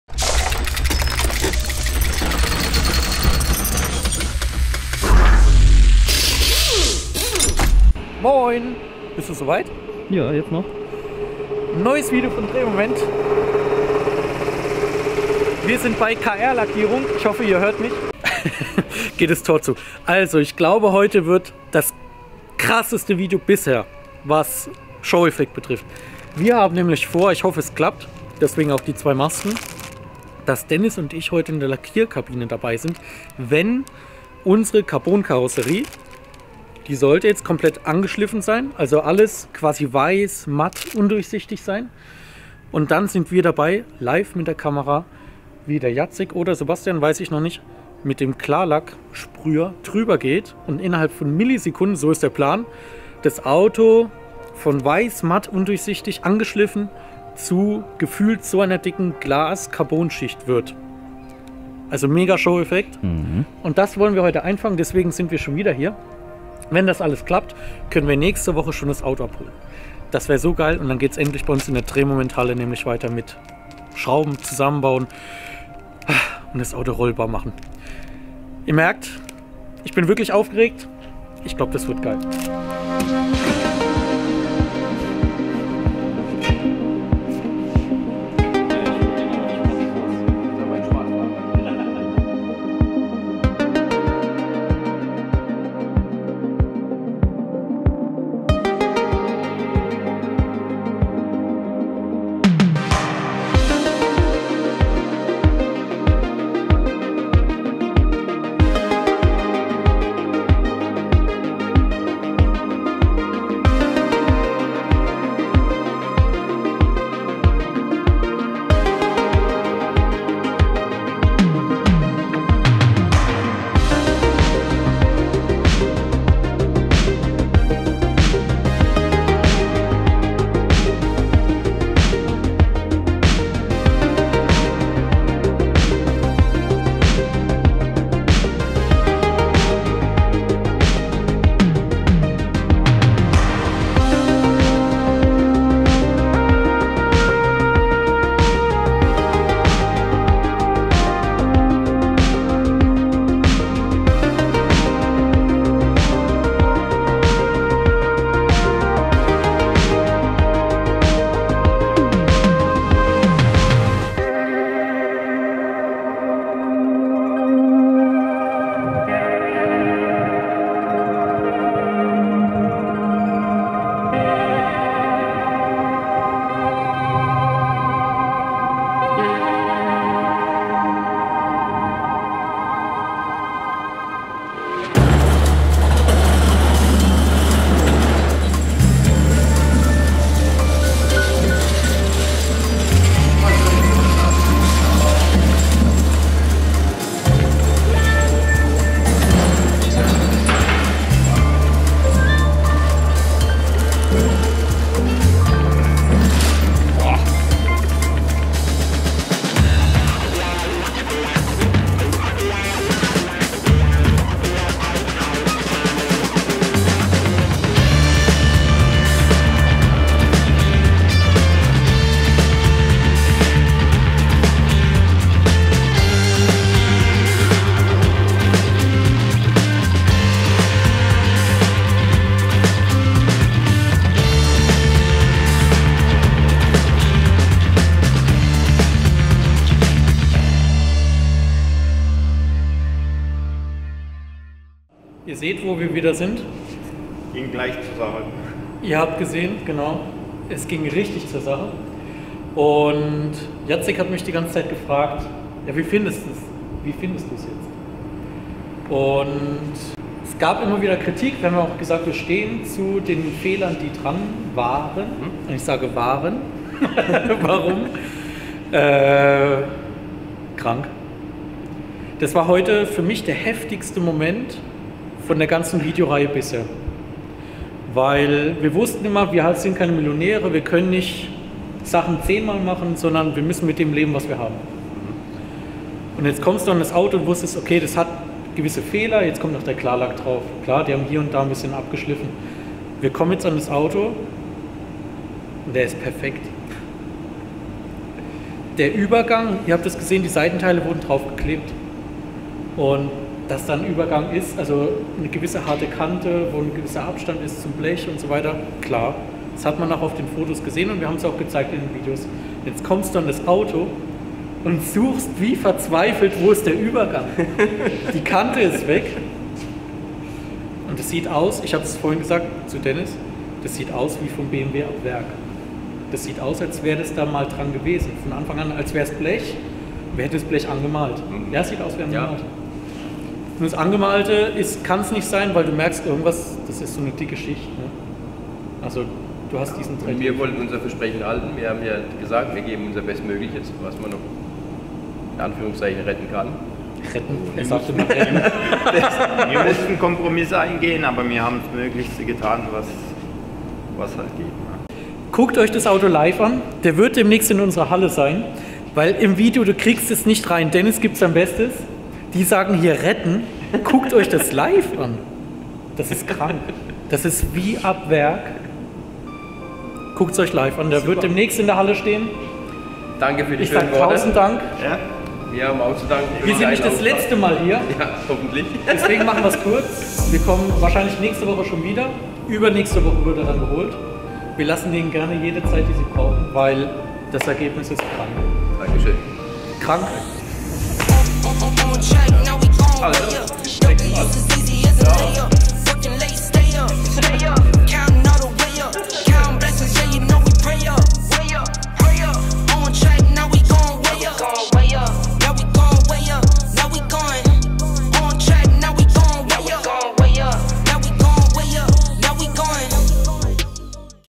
E30 M3 Carbon Lackierung Teil 3 | Bei den Profis von K&R Lackierung | Das Projekt | Episode 26
Dieser Moment, wenn der Klarlack auf die angeschliffene Karosserie trifft! Darauf haben wir gewartet und jetzt sind wir mit Euch "live" in der Lackierkabine dabei!